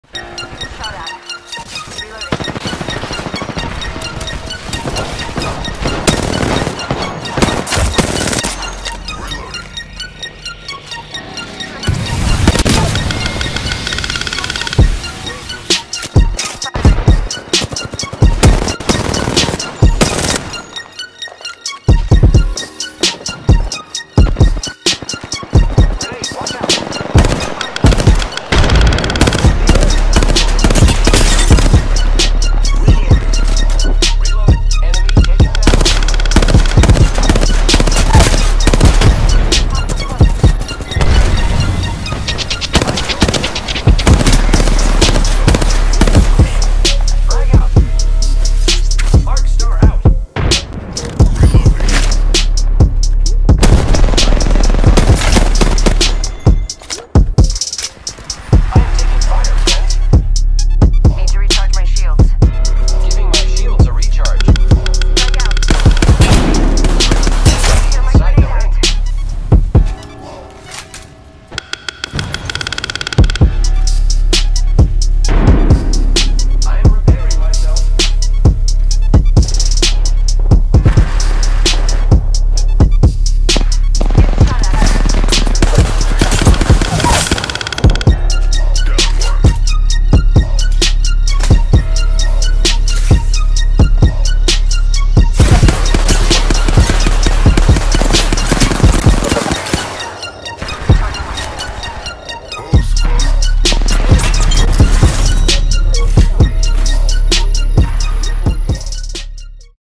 First time on PC I get more than two kills...and the DUB!
First time on PC I get more than two kills...and the DUB!  You never know what you get with ransoms (I’m usually the random).  Sometimes I step up and rise to the occasion.